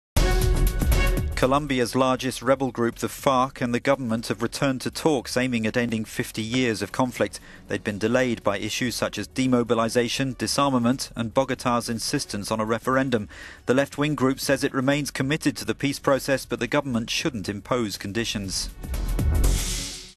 Colombia government and FARC rebels return to peace talks
Colombia's largest rebel group the Revolutionary Armed Forces of Colombia (FARC) and the government have returned to talks aimed at ending 50 years of conflict in the country.

Negotiations been delayed by controversial issues such as demobilisation and disarmament. Bogota's insistence on a national referendum on any final peace accord had also stalled the dialogue.

The left-wing group's chief negotiator Ivan Marquez said on Friday the rebels remained committed to the peace process, but called …